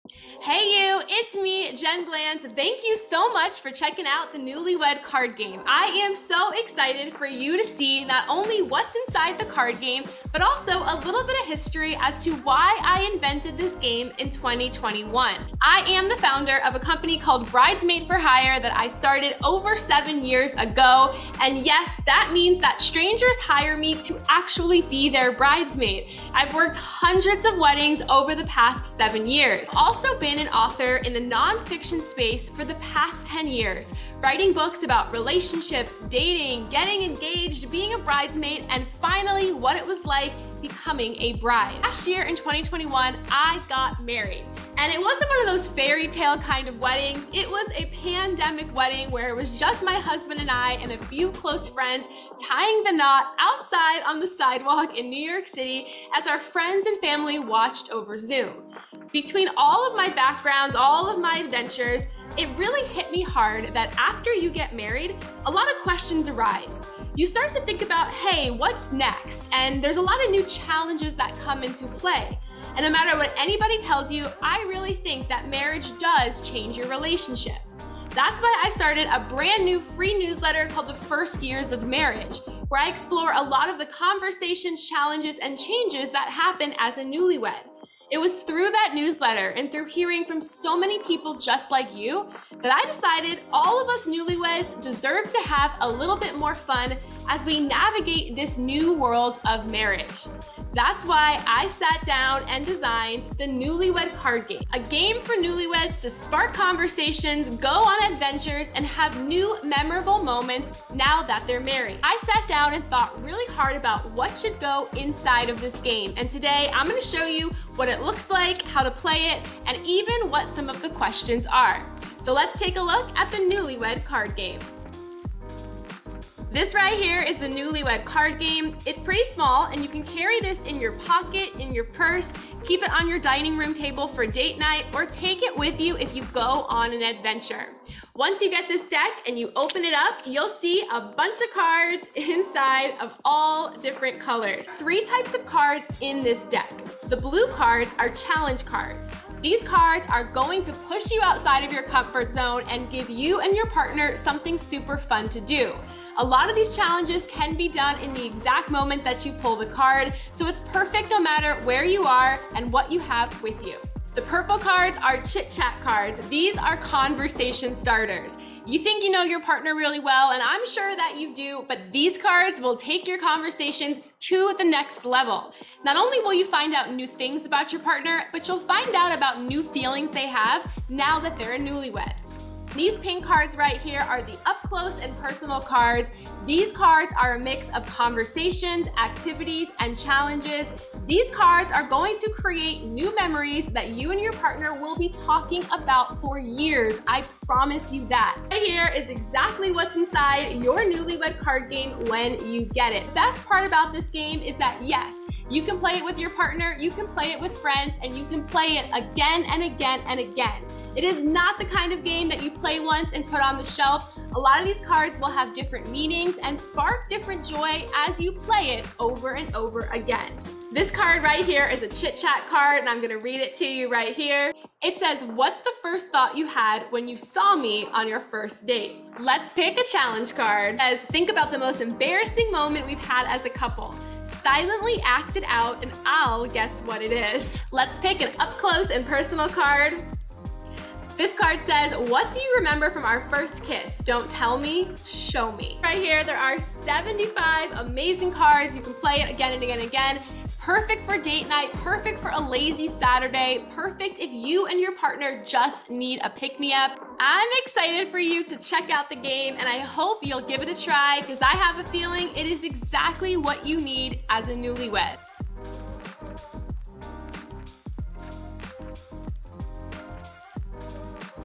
The Newlywed Card Game
Meet the first-ever card game for newlyweds that adds a spark of romance, adventure, and fun after the honeymoon ends.